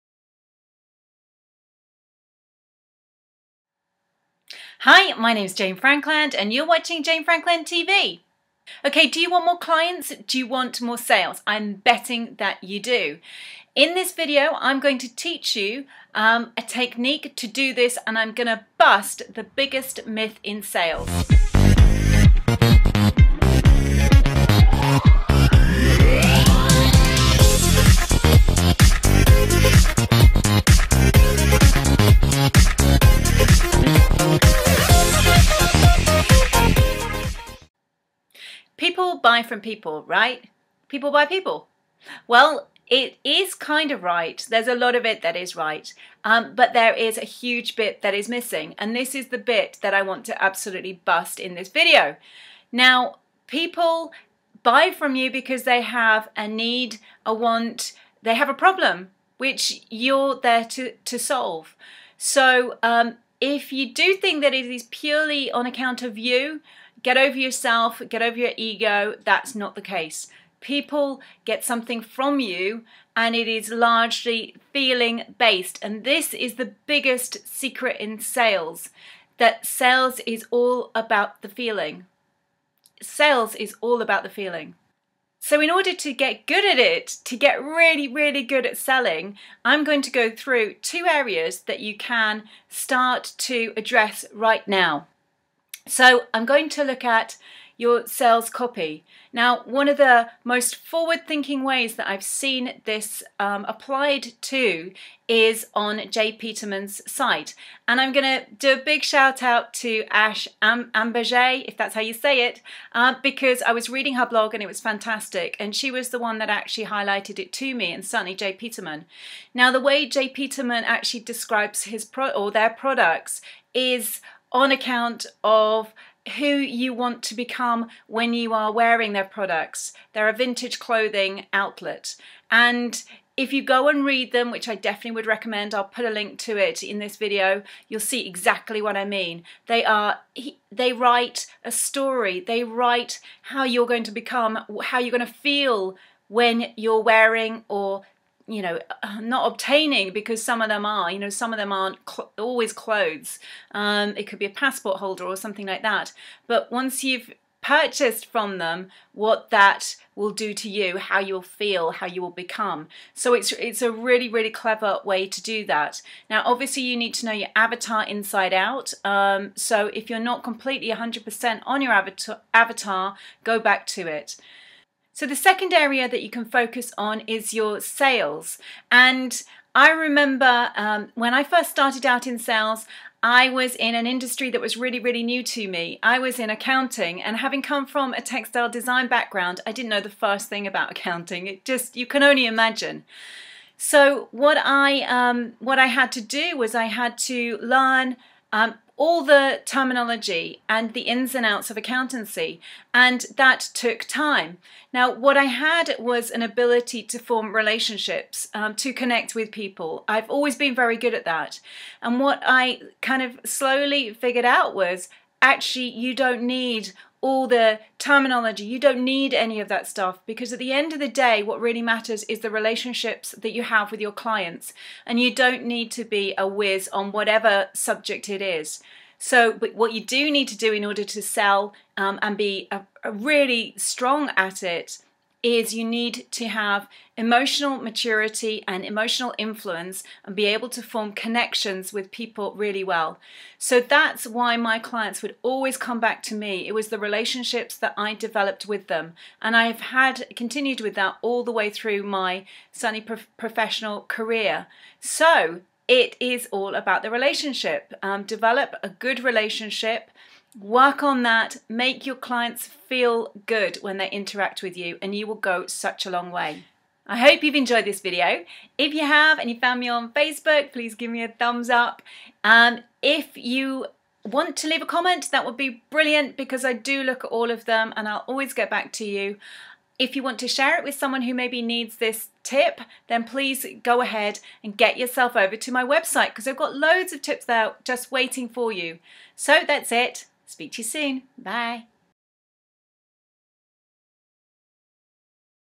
Selling Tips & Sales Advice: The Biggest Myth in Sales Busted
Want more clients? Want more sales? Well lets bust the biggest myth in sales & teach you how to apply a technique that will do this. Get ready to become a winner & up your sales game.